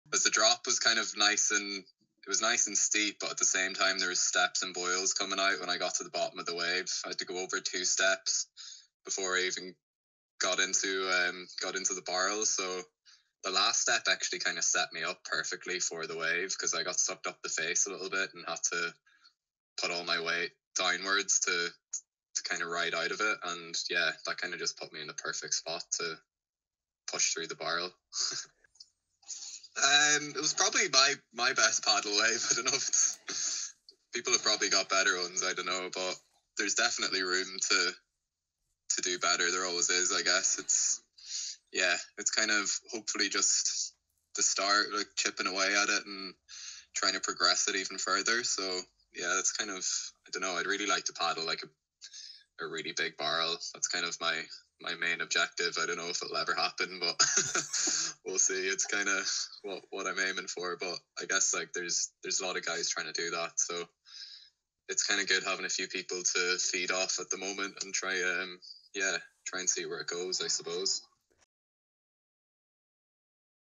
Conor Maguire's Big Paddle Bomb | February 2023
Conor Maguire talks through his best paddle wave ever at Ireland's premier hell slab.

Video: Clem McInerney.